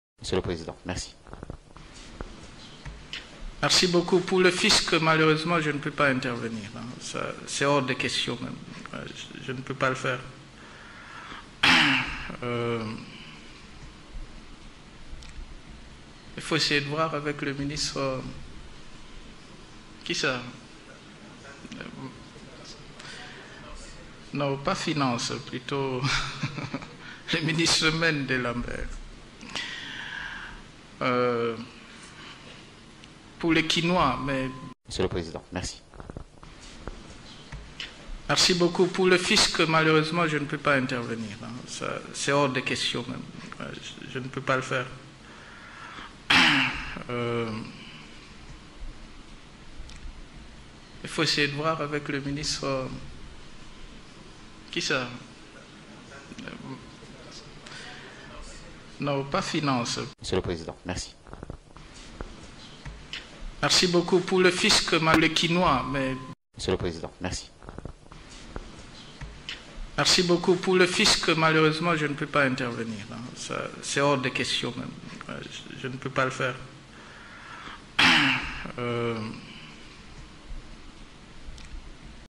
Joseph Kabila ignore le mistère de tutelle du fisc, Ah JP Bemba revient, la RDC est en péril
Lorsque le journaliste sur cet élément sonore qui vous est proposé pose à Joseph Kabila une question sur le fisc, il refuse de repondre et malgré qu'un de ces collaborateurs lui souffle le nom du ministère de tutelle, en l'occurence celui des finances, le chef de l'état s'obstine et refuse la voie de sortie qui s'offre à lui et renvoit la cette question concernant le fisc au ministre Lambert Mende, porte parole du gouvernement. Quel niveau a réellement ce chef d'état? une vraie calamité pour la RDC et ses 70 millions d'habitants. Cgh